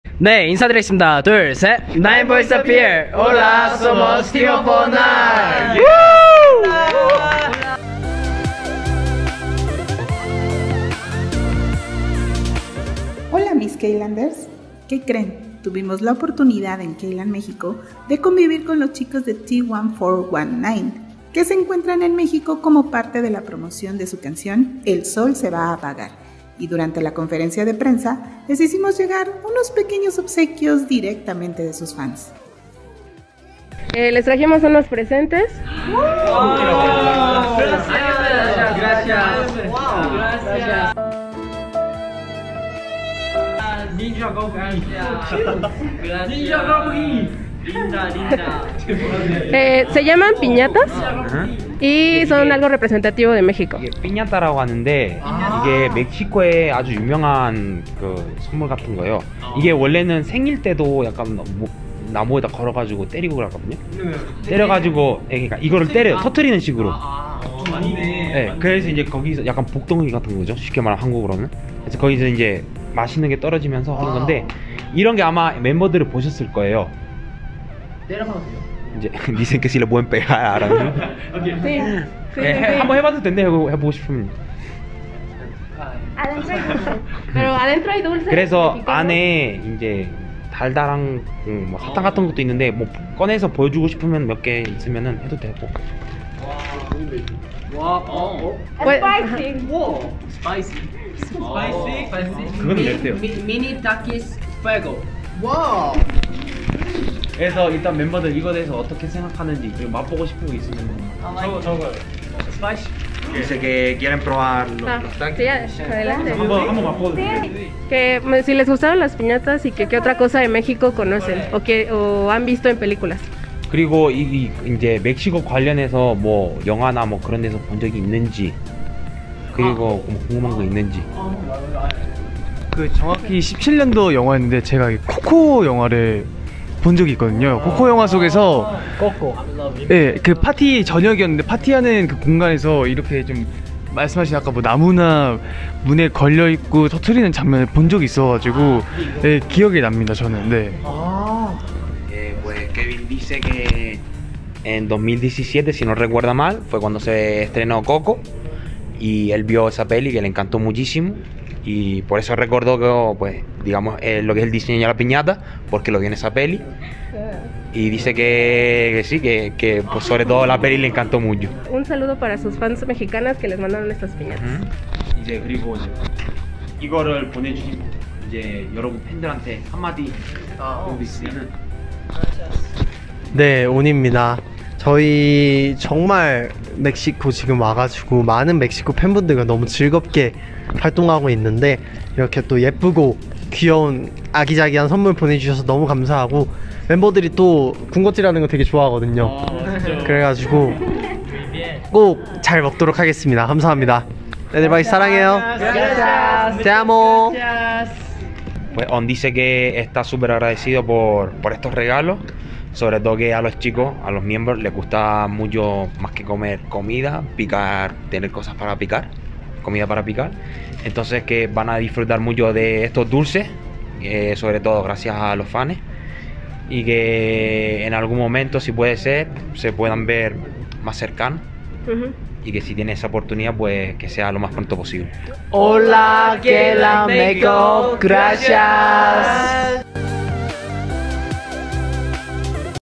[ENG SUB] 220708 T1419 / TFN try Mexican Piñatas! T1419 in Mexico 2022
Original video:
¡T1419 conoce las piñatas mexicanas! / T1419 en Mexico 2022

English subbed by me (sorry for any mistakes)